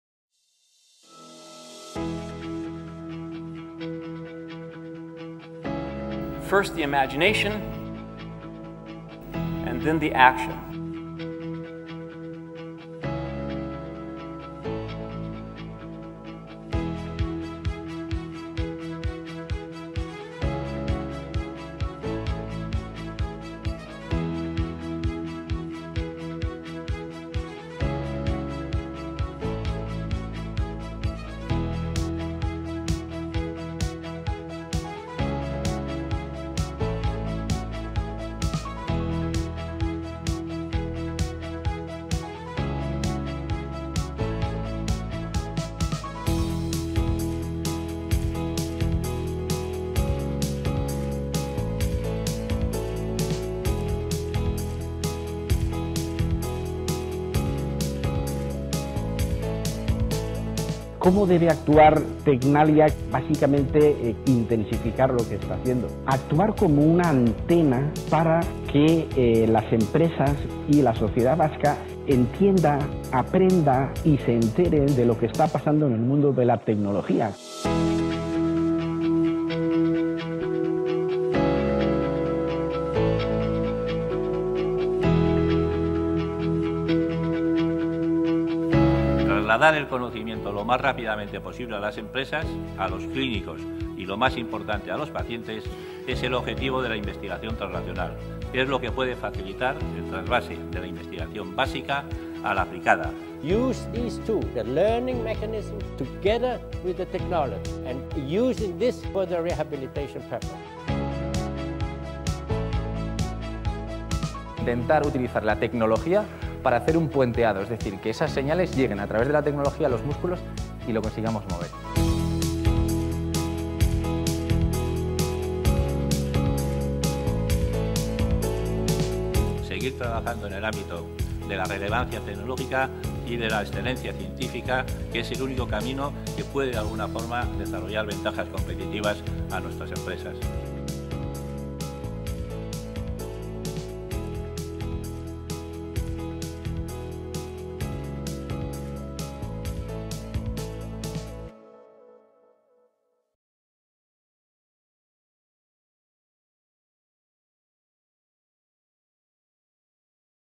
Perspectives2015, cuando industria y tecnología van de la mano
TECNALIA Perspectives 2015. “Industria y Tecnología: Investigación traslacional, de la ciencia al mercado” es el título del evento que contó con la participación del Dr. Niels Birbaumer, experto mundial en el desarrollo de interfaces cerebro-computador y del investigador Dr. Ander Ramos, Premio al Mejor Investigador Joven de Alemania en 2014 por la Academia Alemana para las Ciencias y las Letras.

También presentamos dos novedosas iniciativas de TECNALIA como son un dispositivo de estimulación eléctrica funcional para rehabilitación de pacientes de ictus y el robot quirúrgico con visión 3D y sensaciones táctiles.